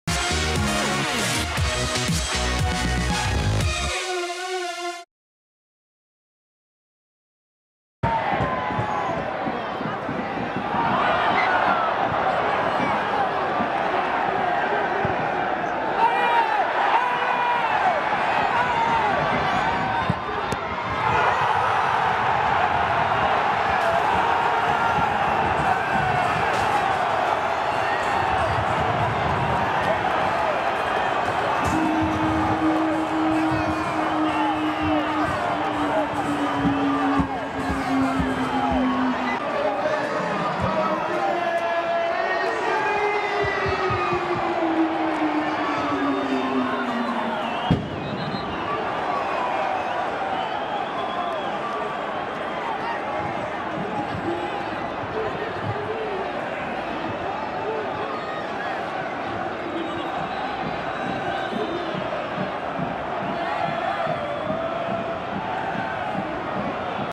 Goal Valentin EYSSERIC (82') - OGC Nice - FC Lorient (1-1) / 2012-13
OGC Nice vs FC Lorient (1 - 1) : Valentin EYSSERIC (82') goal. All OGC Nice vs FC Lorient goals in video.

Stade du Ray - Sat, 09 Feb 2013

Goals : Lucas MAREQUE (75' - FC Lorient) / Valentin EYSSERIC (82' - OGC Nice)